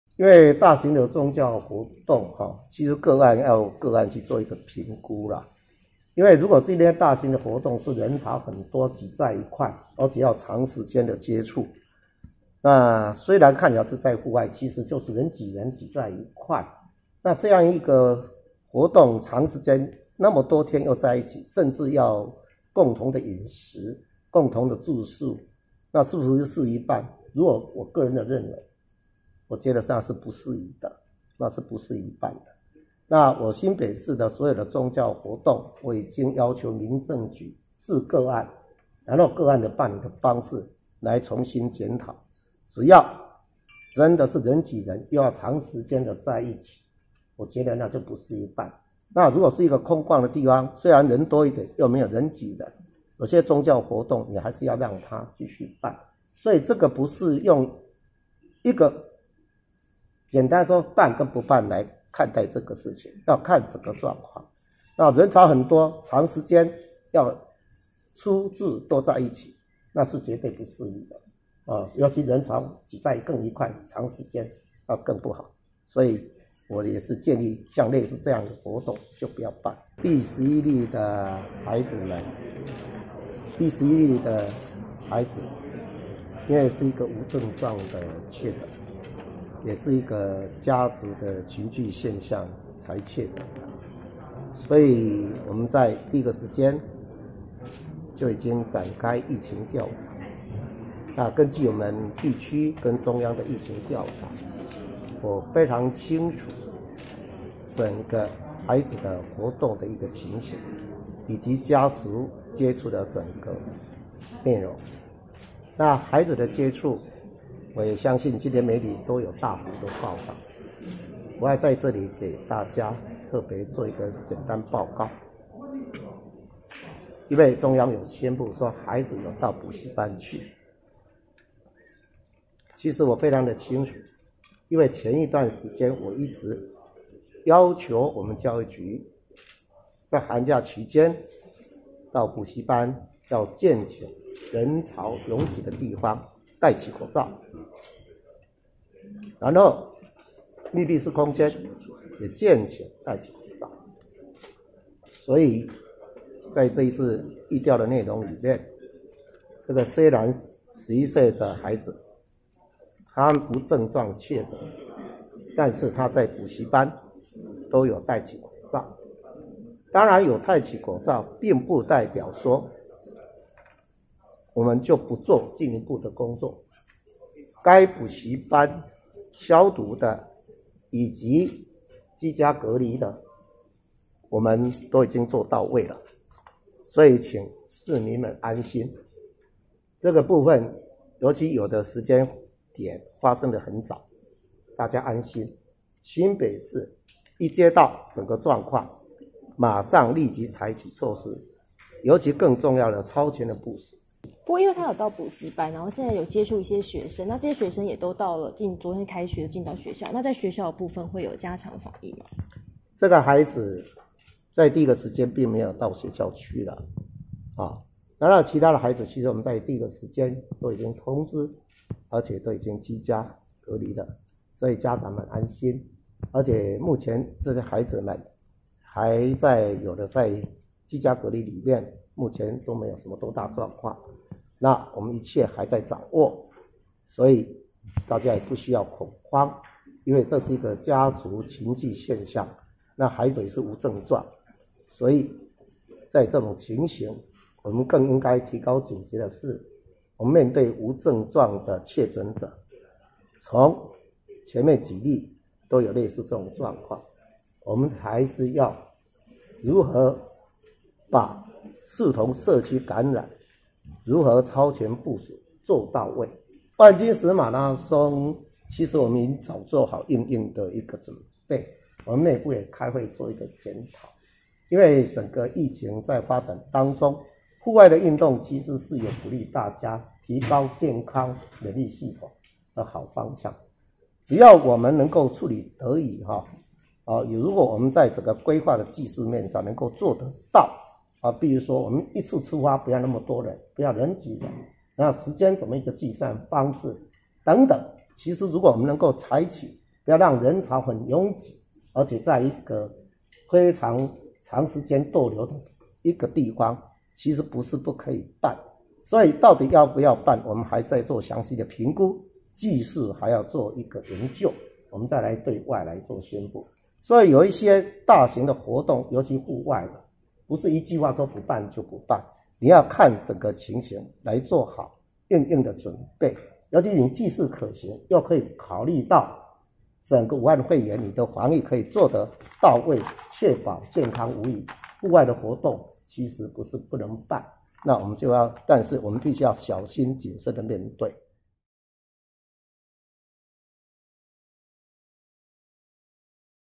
武漢肺炎》大甲媽遶境照常舉行 侯友宜：不適宜
新聞連結：
(04:26)武漢肺炎》萬金石馬拉松取消？侯友宜：評估中
(01:20)新北11歲男童無症狀確診 侯友宜：有戴口罩、補習班消毒

大甲鎮瀾宮董事長顏清標今天宣布，大甲媽祖遶境活動將照常舉行。新北市長侯友宜表示，針對人擠人、長時間接觸的活動，且共同住宿、飲食，他認為不適宜，新北市的宗教活動將視個案重新檢討是否舉辦。